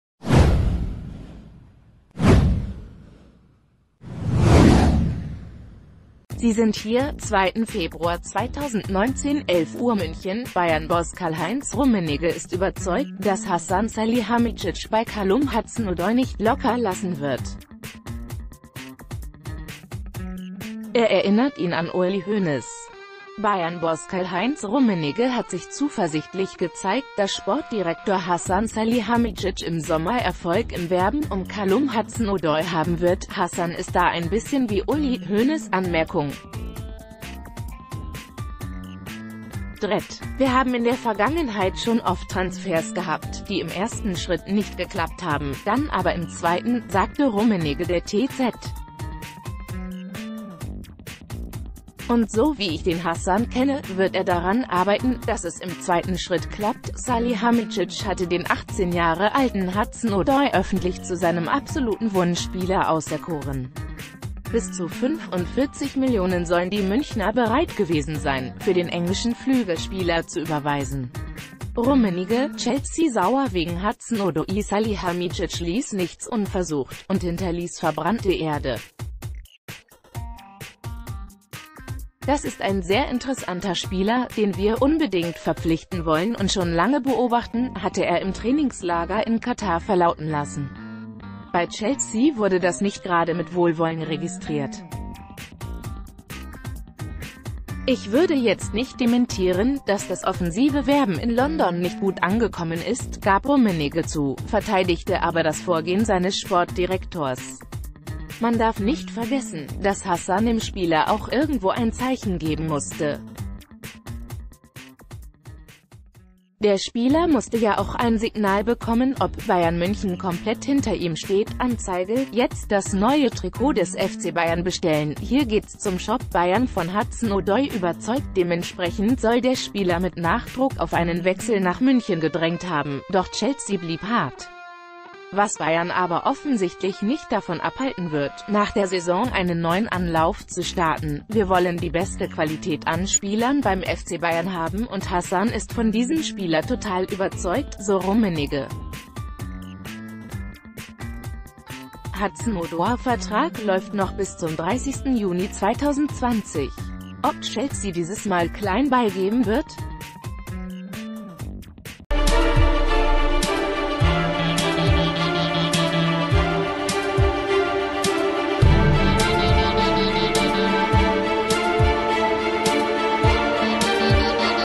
FC Bayern: Rummenigge vergleicht Salihamidzic mit Hoeneß
Sie sind hier: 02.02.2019 / 11:00 Uhr München - Bayern-Boss Karl-Heinz Rummenigge ist überzeugt, dass Hasan Salihamidzic bei Callum Hudson-Odoi nicht locker lassen wird. Er erinnert ihn an Uli Hoeneß. Bayern-Boss Karl-Heinz Rummenigge hat sich zuversichtlich gezeigt, dass Sportdirektor Hasan Salihamidzic im Sommer Erfolg im Werben um Callum Hudson-Odoi haben wird."Hasan ist da ein bisschen wie Uli (Hoeneß, Anm. d. Red.). Wir haben in der Vergangenheit schon oft Transfers gehabt, die im ersten Sc...